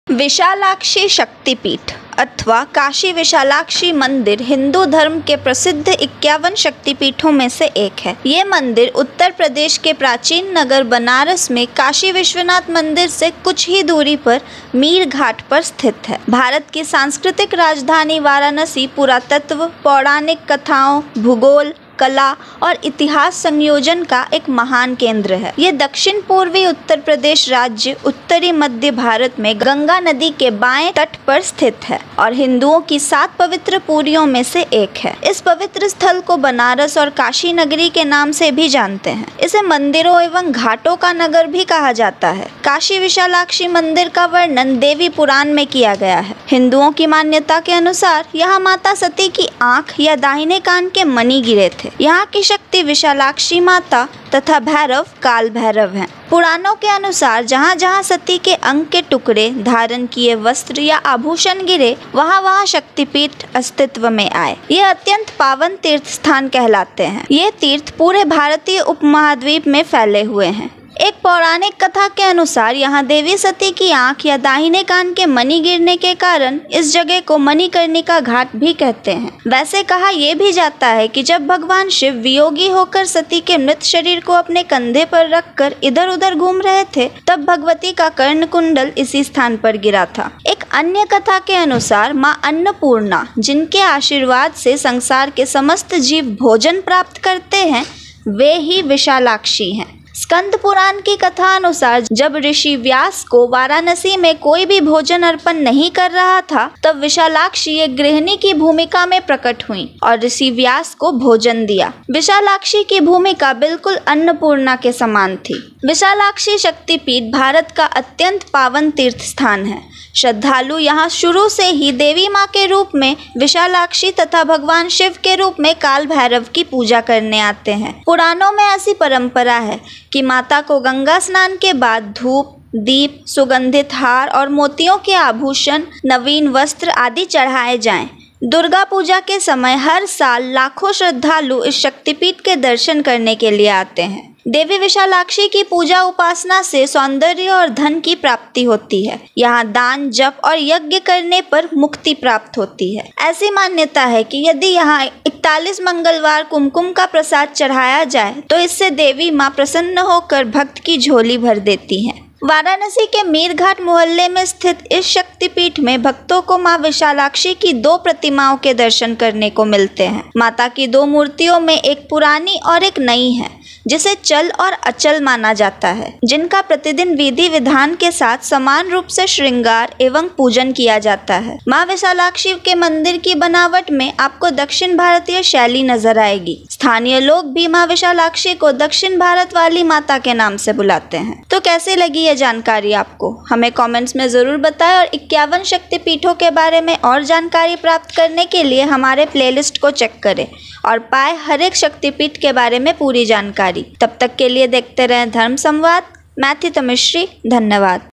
विशालाक्षी शक्तिपीठ : यहाँ गिरे थे माता सती के 'दाहिने कान के मणि' | Vishalakshi Shaktipeeth
हमसे जुड़े और अपने धर्म को जाने.